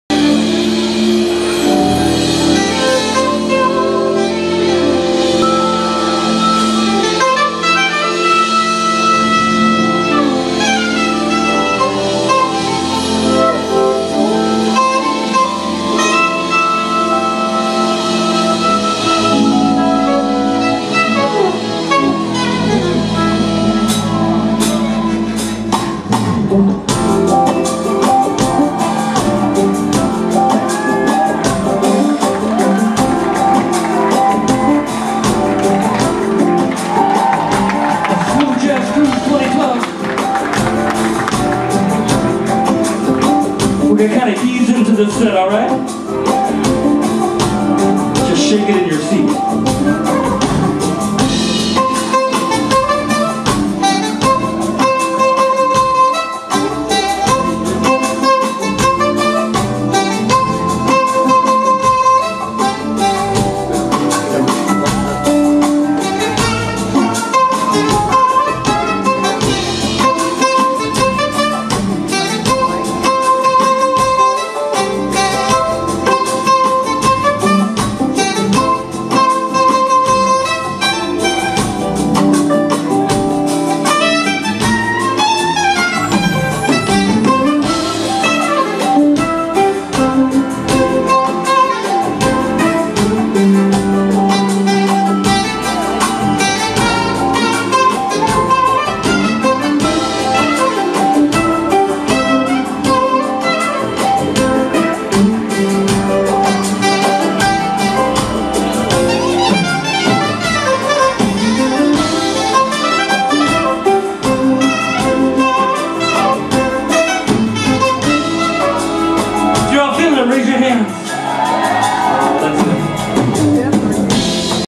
Euge Groove performs on The Smooth Jazz Cruise.mp4
Euge Groove performs aboard The Smooth Jazz Cruise on the Holland America Westerdam.